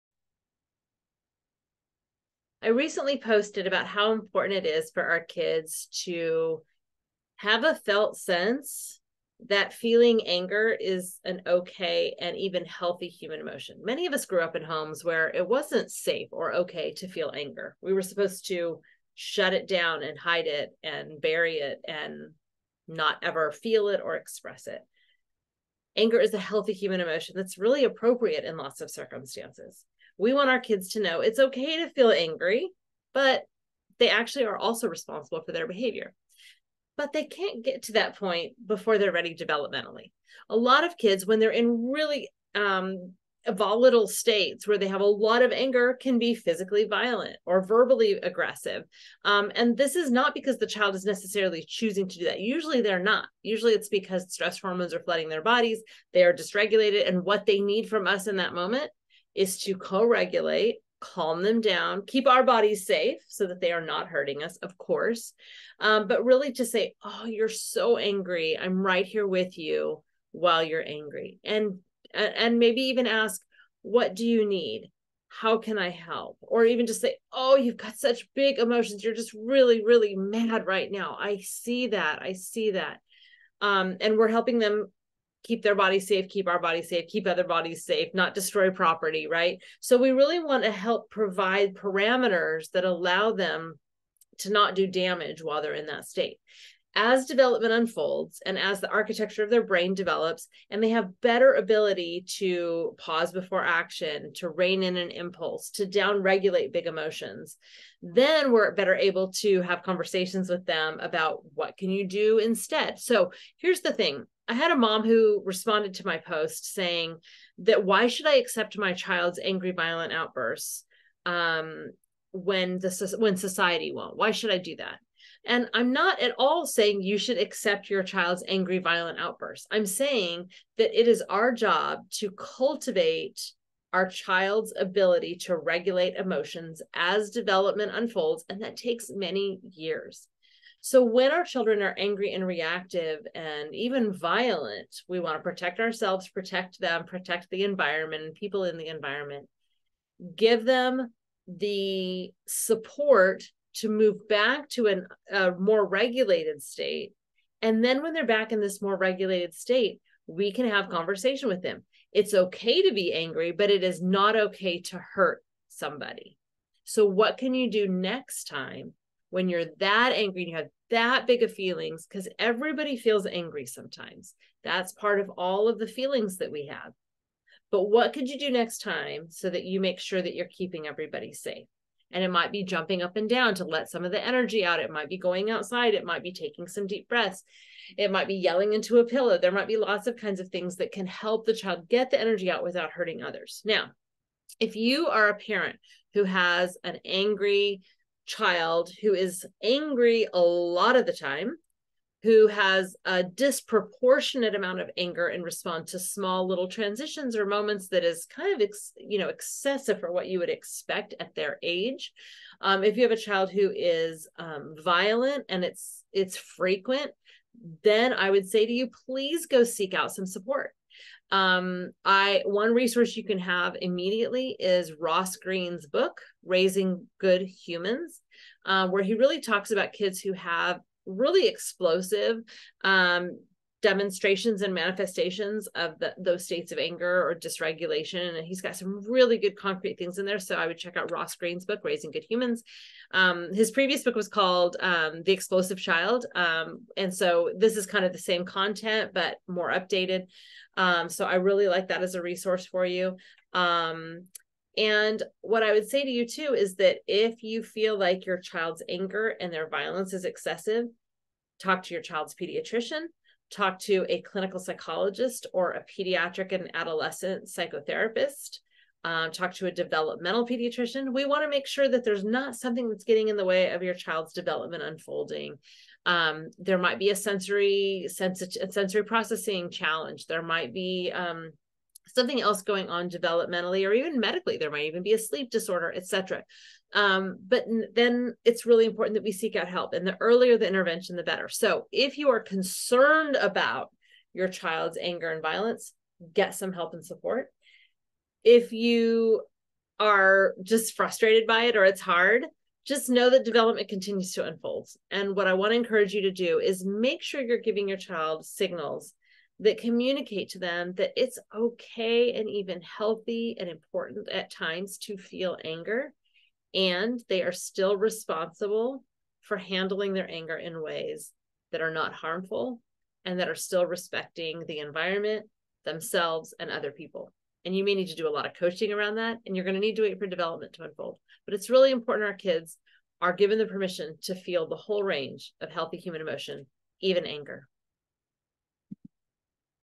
VLOG: How to Handle Angry Kids from Tina Bryson
How should we respond when our kids are angry? Instead of shutting them down, getting angry back at them, or taking it personally, here’s how we can effectively hold boundaries and support our kids when they’re angry.

We want to respond in ways that let our kids know that anger is a healthy emotion that is safe to express, but we also want to teach them how to express it appropriately.